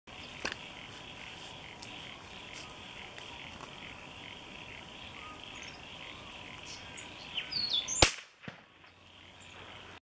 rabbit hunting multiple kill shots
Nothing quite like sneaking up on a rabbit and shooting him with the .22 and subsonics.  Good fun during the middle of the day whilst the sun is out.